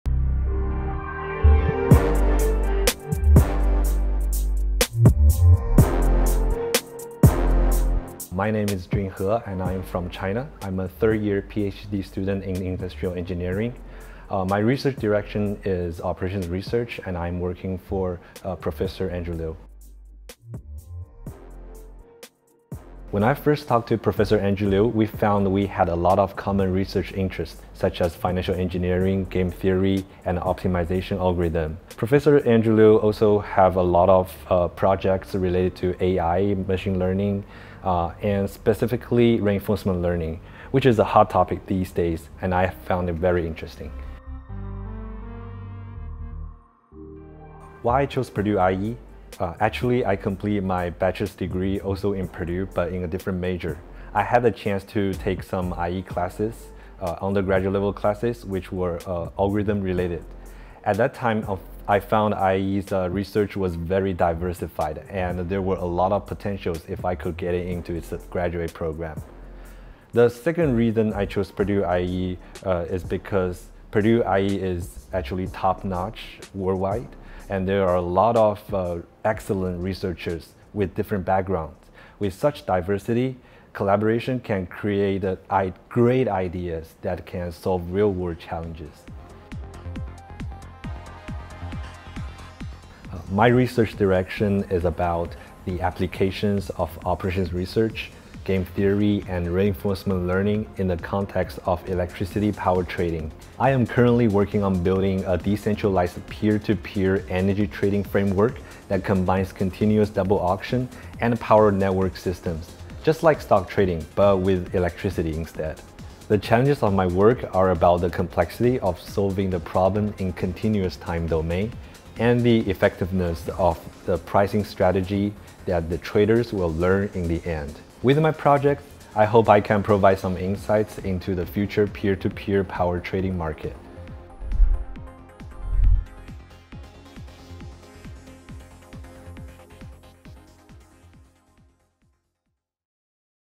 Student Spotlight - PhD Student Jun He
Jun He is a 3rd year PhD student. Watch this quick video to learn about operations research at Purdue from his perspective!